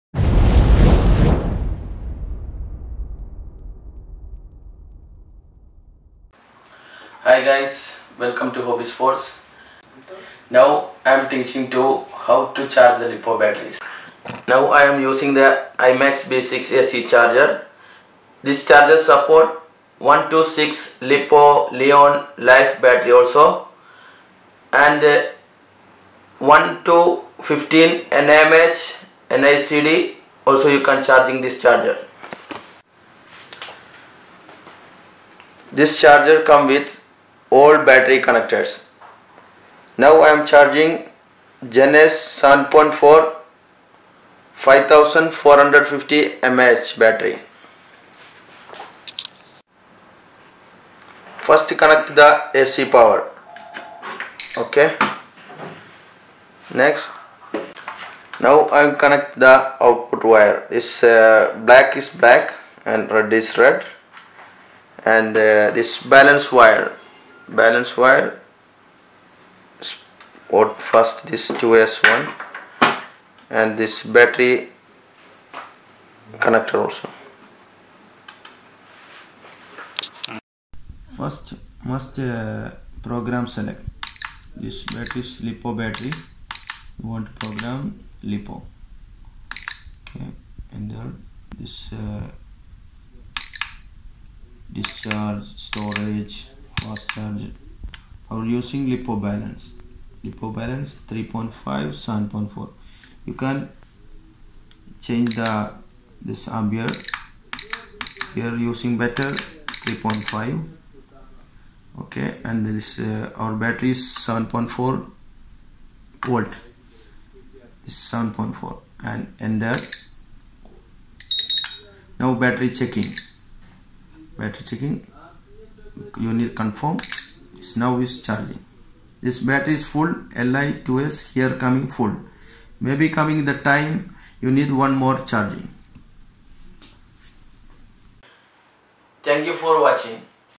How to Charge Lipo battery 2s to 6s-imax b6 and gens ace battery-Hobby sportz
Charger : Imax b6 AC Charger/Discharger
Battery  : Gens Ace 2s1p 7.4v 45c 5450mAh
Connector : Traxxas
we are used Balance Charging mode to charge this battery.because balance charging mode detect the Voltage of the battery.
its helps to extend the battery life.